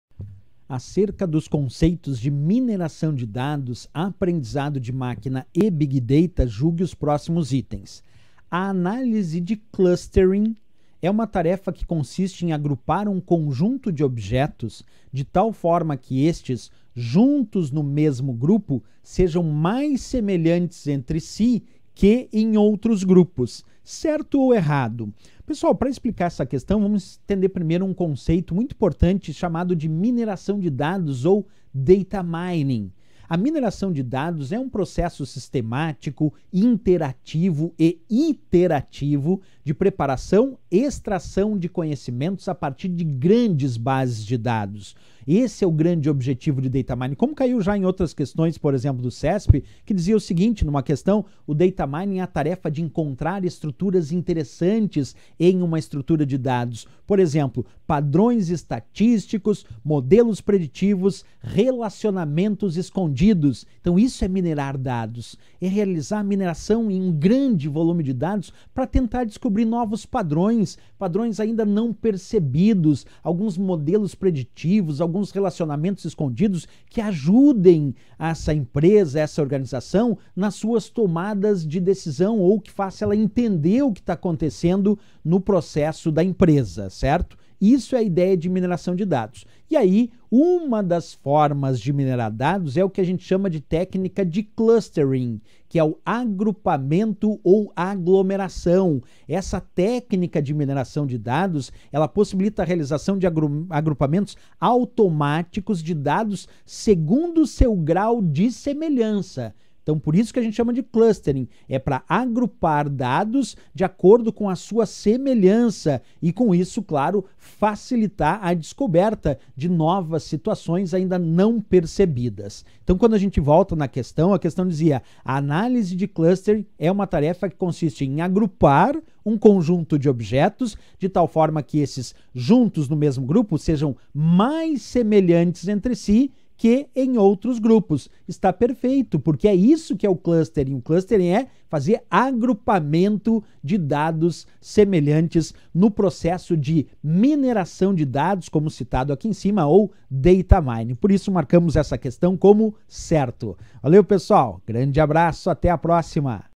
Concurso Polícia Federal 2021 | CORREÇÃO QUESTÕES INFORMÁTICA #27
Vídeo 27 com a correção das questões de informática do concurso da Polícia Federal 2021. Professor Rodrigo Schaeffer.
Questão sobre: Mineração de dados e o clustering.